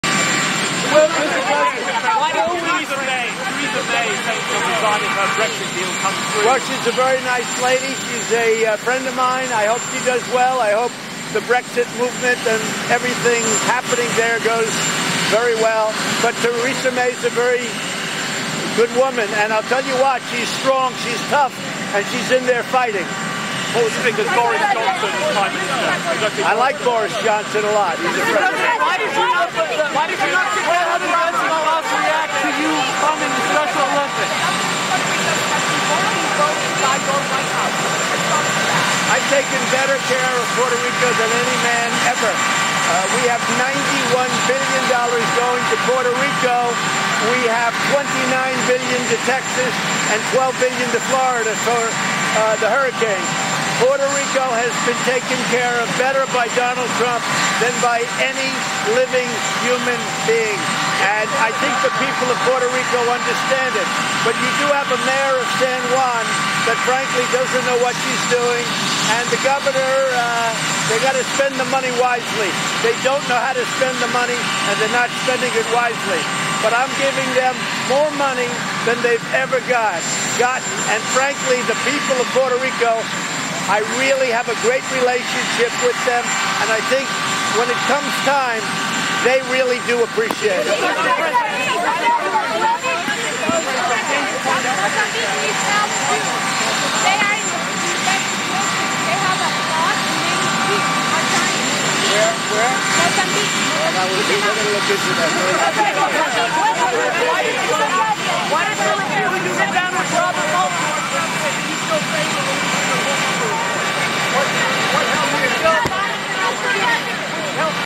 Trump Claims That He's 'Taken Care of Puerto Rico Better Than Any Man Ever'
"I have taken better care of Puerto Rico than any man ever. "